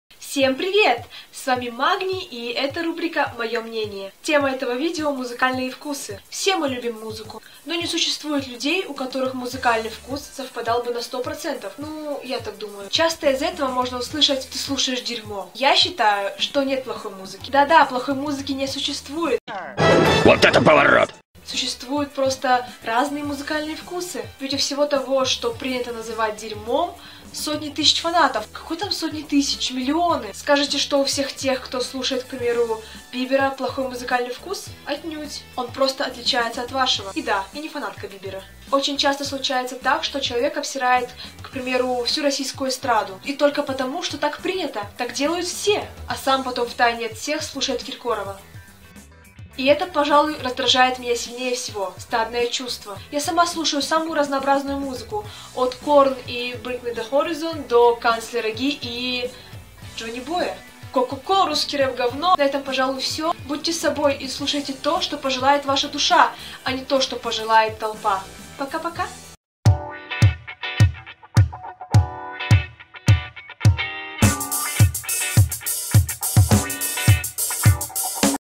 МУЗЫКАЛЬНЫЕ ВКУСЫ
► МУЗЫКАЛЬНЫЕ ВКУСЫ [Мое мнение] ◄

Это видео о такой наболевшей теме как "музыкальные вкусы".
Это мое субъективное мнение, я никому его не навязываю.